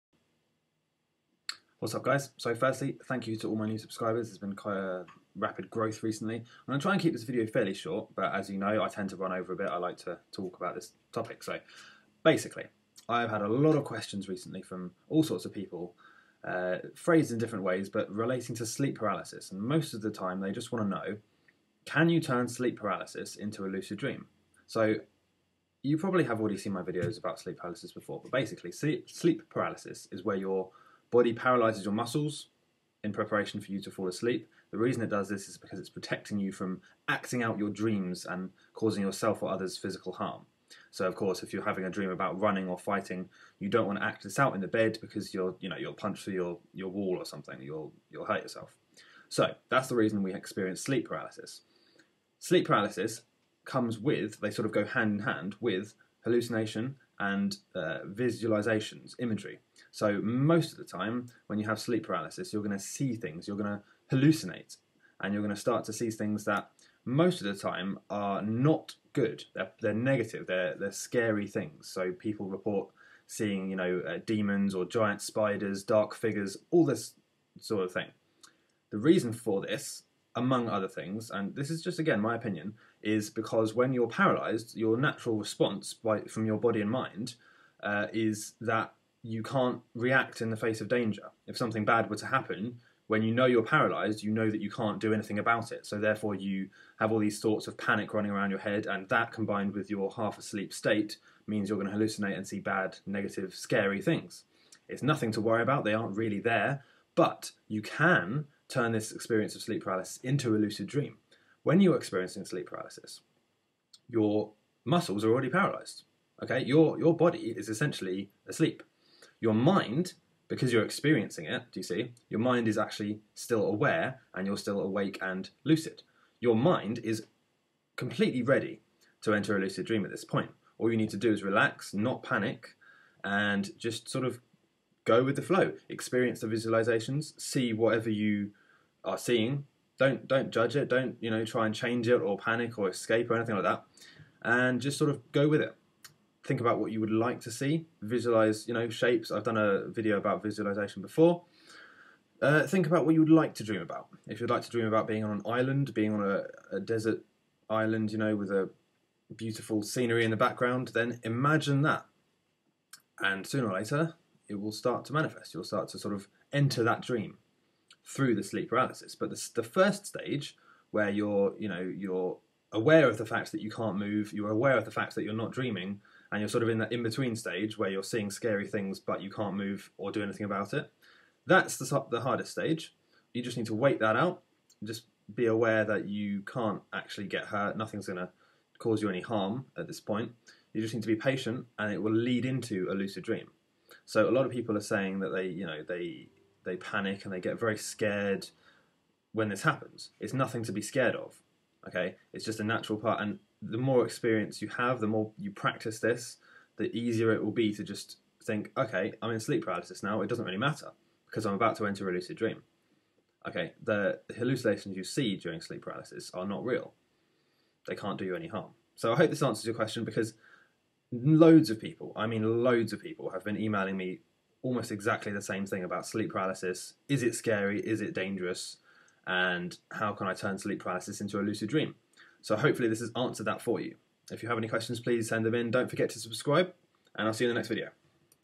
Why Sleep Paralysis Is A GOOD Thing (Not A Scary Thing)
✨ As a response to a lot of questions about sleep paralysis, I've made a video that should help all of you. Sleep paralysis is actually a good thing! Don't be scared of it.